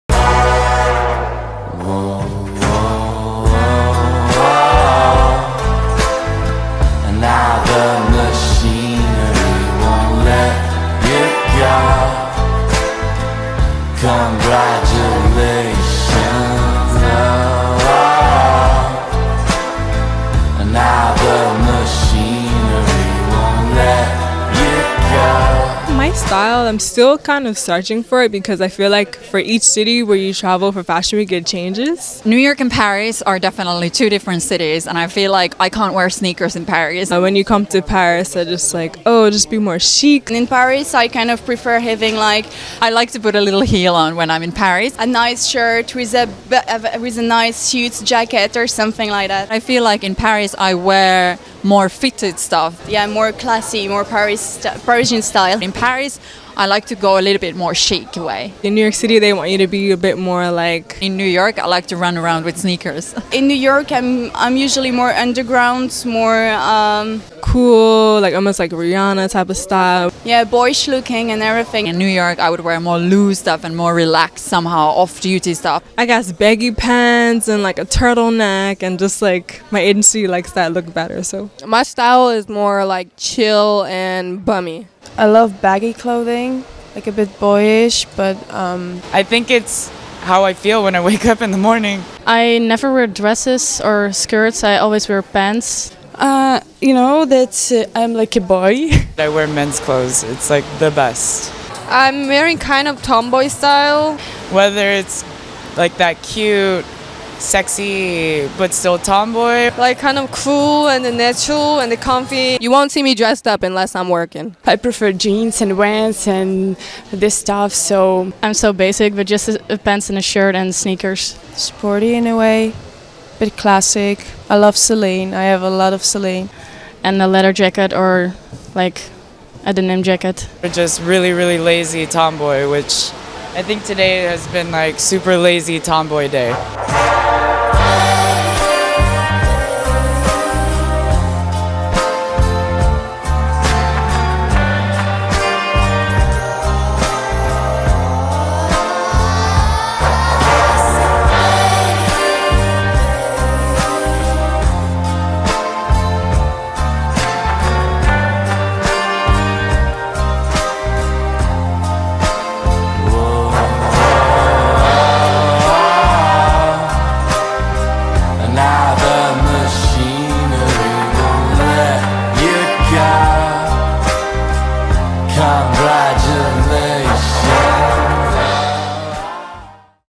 Model Talks My Style Part Two | FashionTV
WORLD - Ever wondered what top runway models like to wear? Get an exclusive inside look as to what models Lily Lightbourn, Yumi Lambert, Saara Sihvonen, Selena Forrest, Sharina Gutierrez and others have to say about their favorite style! Also find out what the models have to say about the difference between dressing in New York versus dressing in Paris!